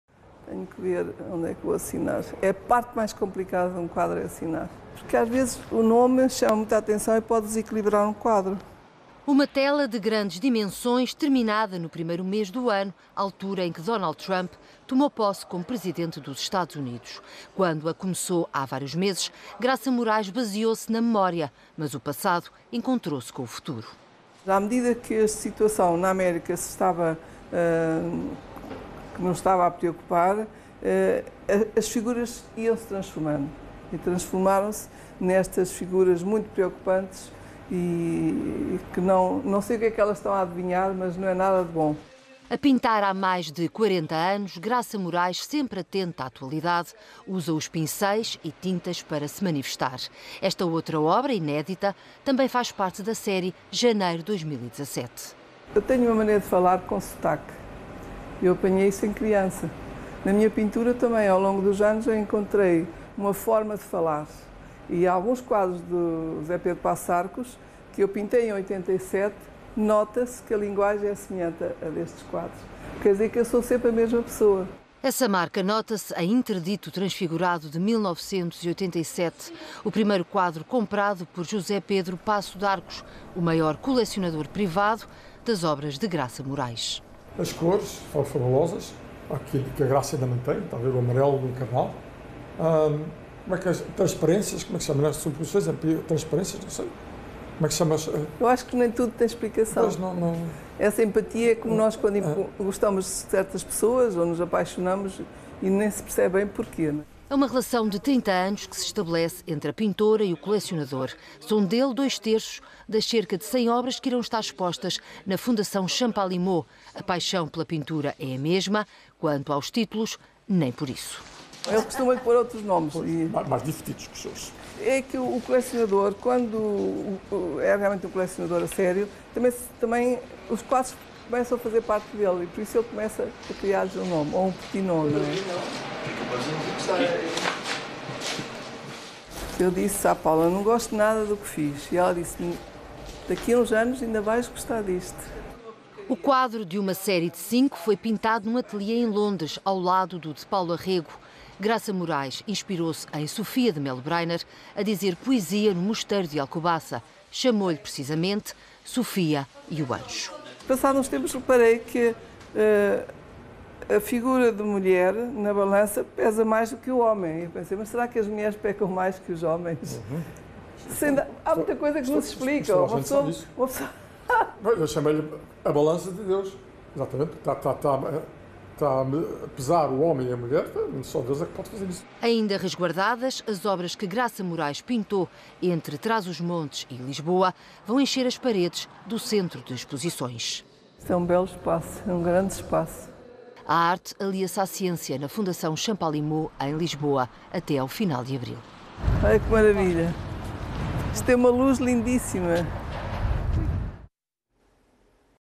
Obras de Graça Morais em exposição na Fundação Champalimaud
Uma exposição de Graça Morais é inaugurada esta quarta-feira, na Fundação Champalimaud, em Lisboa. Cerca de 100 obras da pintora podem ser vistas até ao fim de Abril, na Fundação. A artista pintou dois quadros especificamente para esta exposição.

SIC Notícias, 28 Fev, 2017